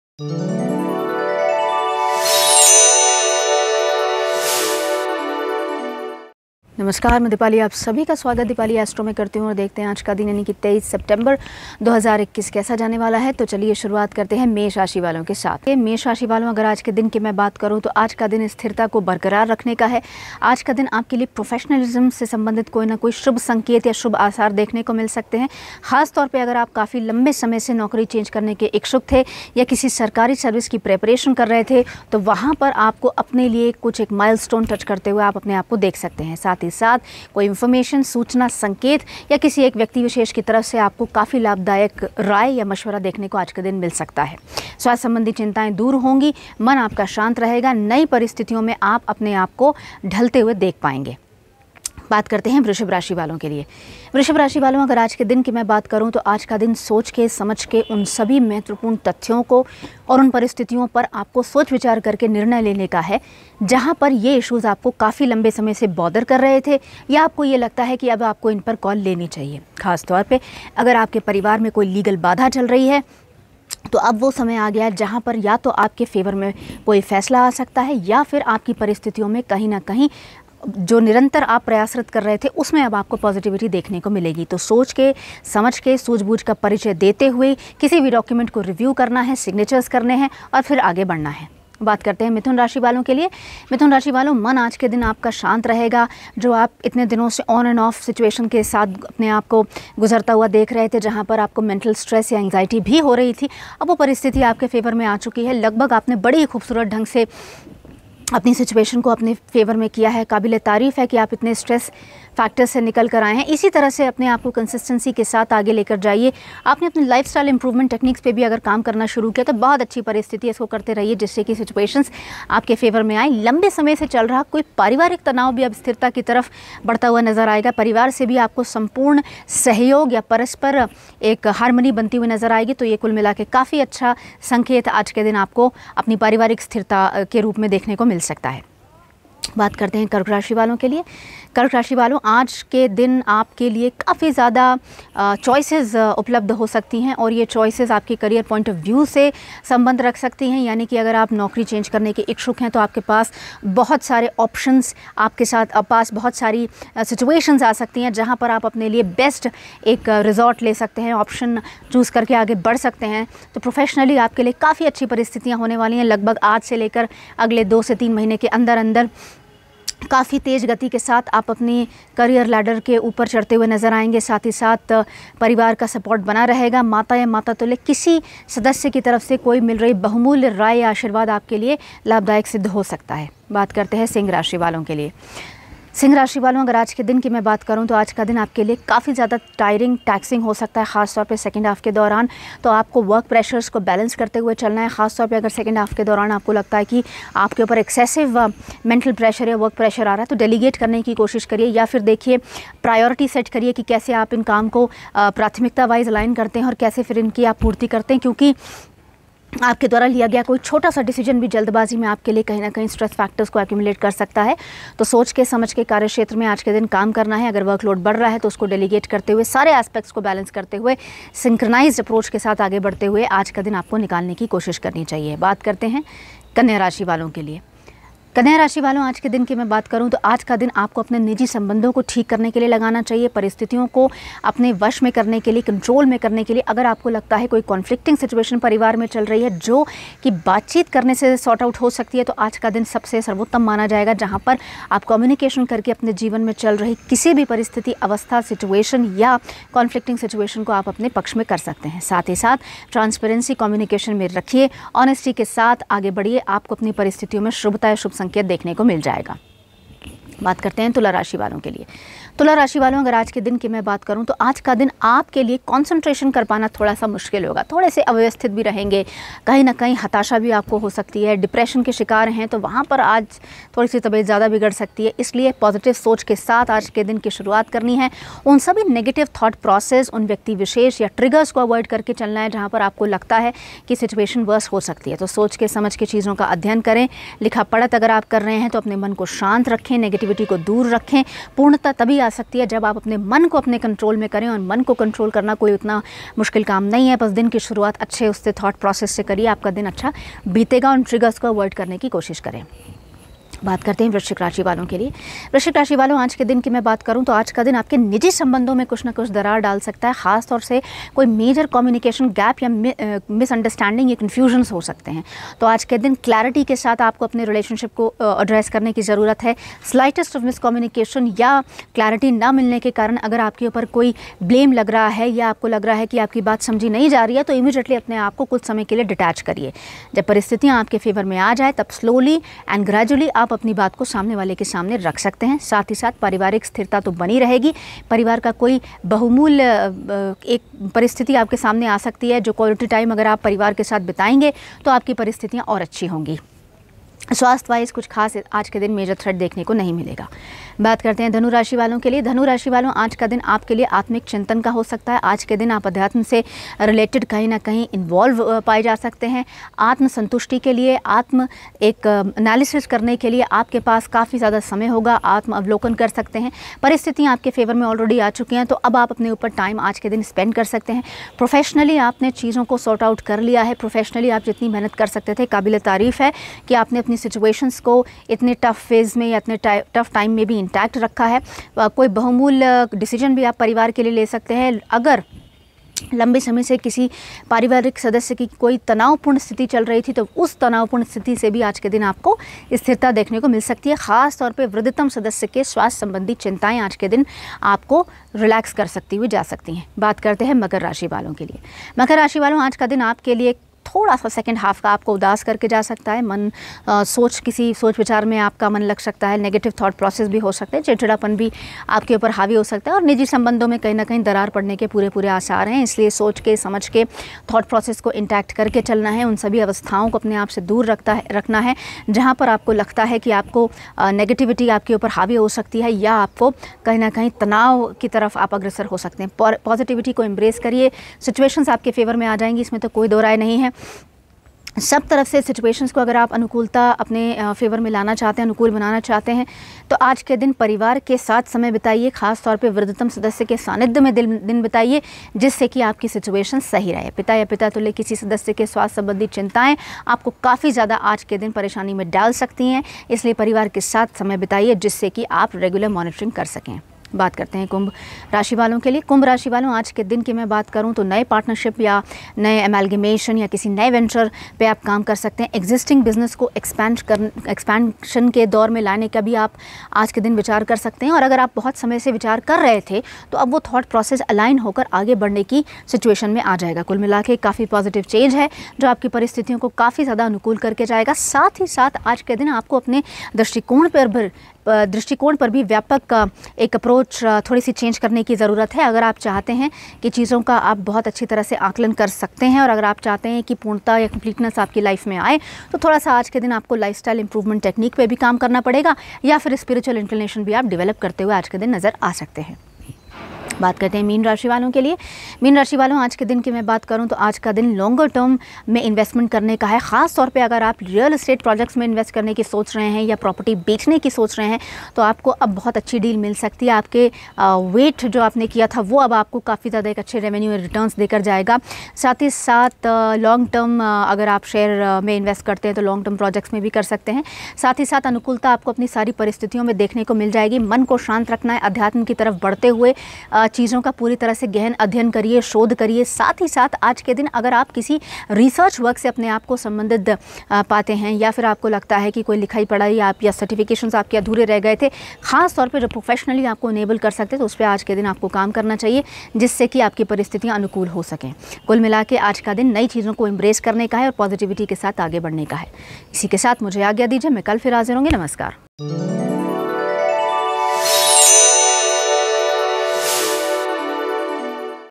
23 September 2021 AAJ KA RASHIFAL | आज का राशिफल मेष से मीन तक | Daily Astrology | Boldsky
Aaj Ka Rashifal 23 September 2021 or Dainik Rashifal by Astrologer Deepali Dubey. In the video, Deepali Dubey will help you to understand the day to day astrology as per zodiac signs. Dainik Rashifal will help you to know the Daily Bhavishyafal or Horoscope. So, before stepping out of your house must watch Dainik Rashifal to avoid any unwanted happenings. You may follow her and contact her : -

आज का राशिफल में 23 सितंबर 2021 के दिन कैसा रहेगा सभी राशि वालों के लिए जानें ज्योतिष दिपाली दूबे जी है । इस वीडियो में ज्योतिषी दिपाली दूबे जी आपको बताएंगी आपके दैनिक राशिफल के बारे में रोजाना रात 10 बजे । सभी 12 राशि वाले जानें ज्योतिषी दीपाली दुबे से हर दिन की भविष्यवाणी ।